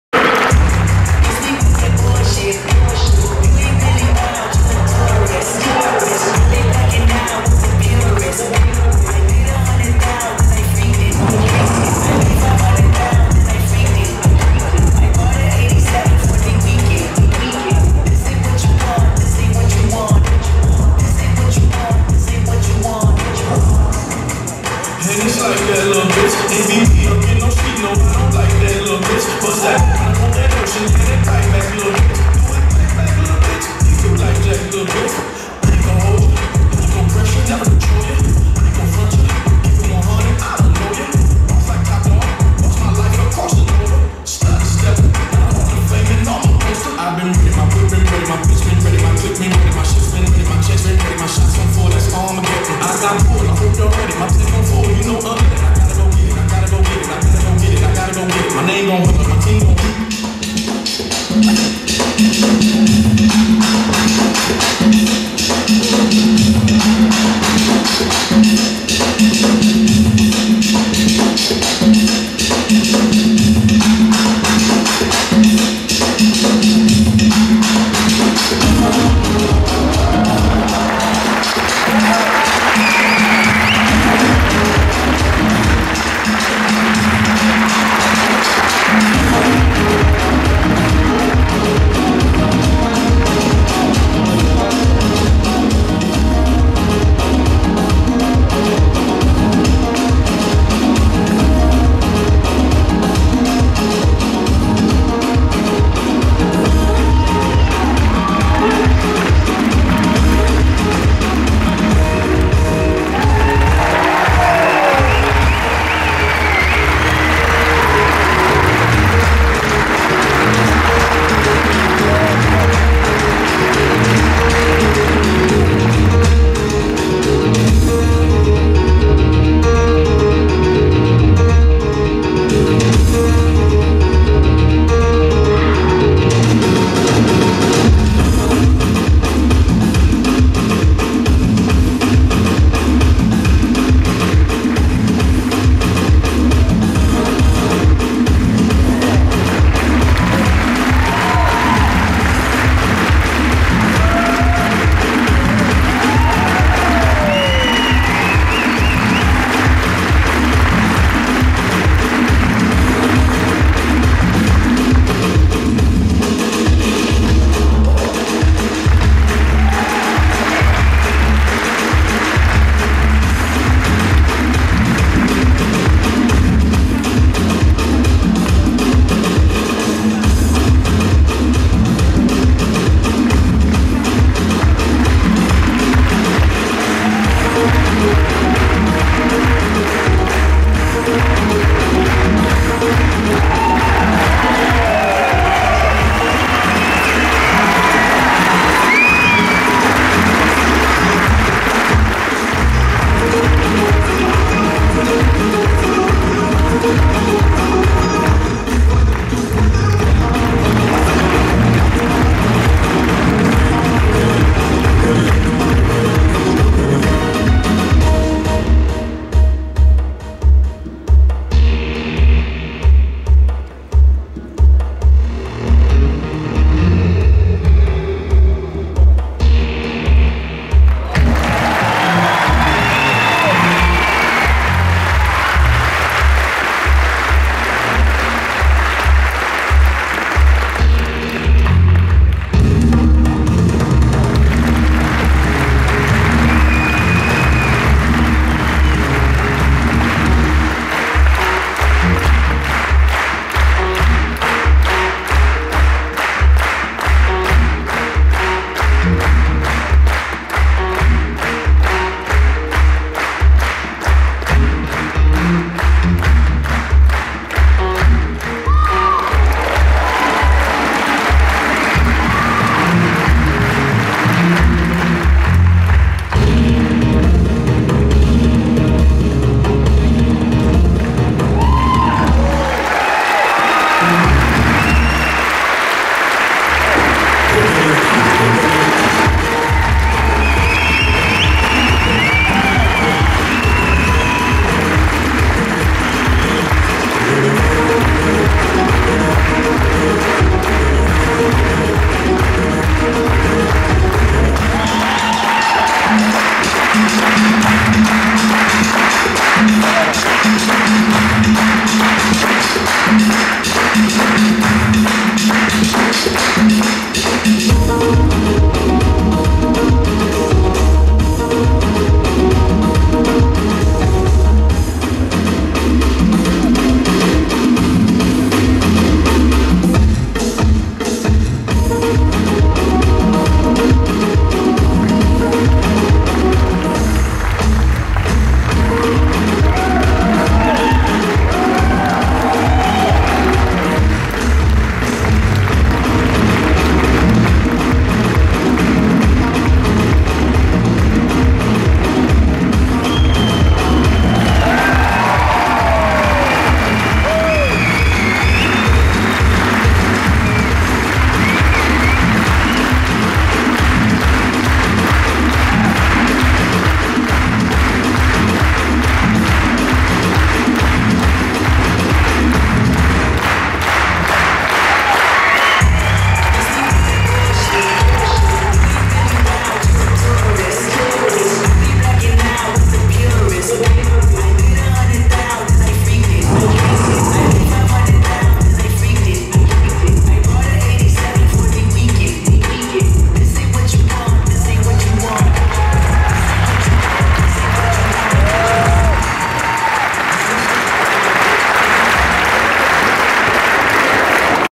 Icarian Games Meda & Miki created by Dmytro Turkeiev at circus Flic Flac
Icarian Games Meda & Miki created by Europhoria agency ( Petar Stoyanov Dmytro Turkeiev ) at circus Flic Flac.